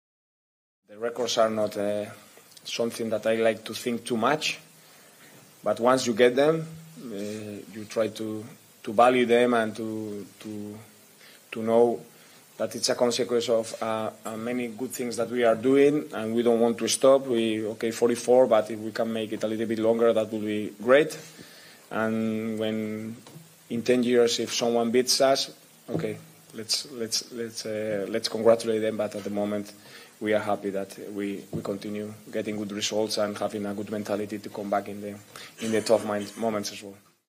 RUEDA de PRENSA de XABI ALONSO tras la CLASIFICACIÓN a SEMIFINALES | WEST HAM VS BAYER LEVERKUSEN
Xabi Alonso analizó en rueda de prensa el empate de su equipo ante el West Ham que le dio el pase a las semifinales contra la Roma.